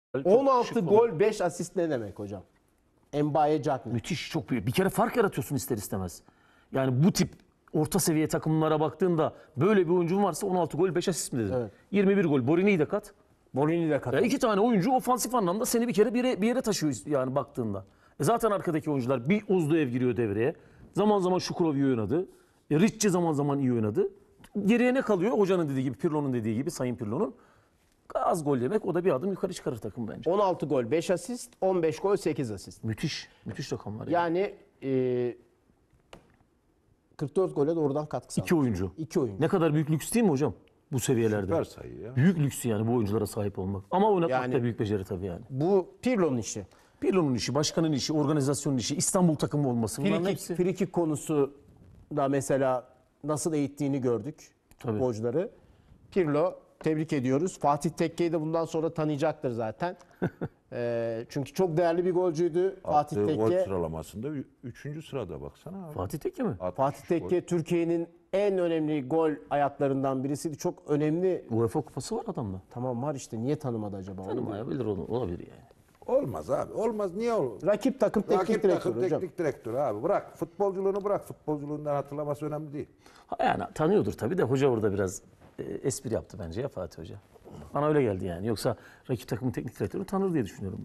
Tarık Üstün: "Borini ve Diagne’den 44 gole doğrudan katkı. Bu oyunculara sahip olmak büyük lüks."
Tarık Üstün: "Borini ve Diagne’den 44 gole doğrudan katkı. Bu oyunculara sahip olmak büyük lüks."futbol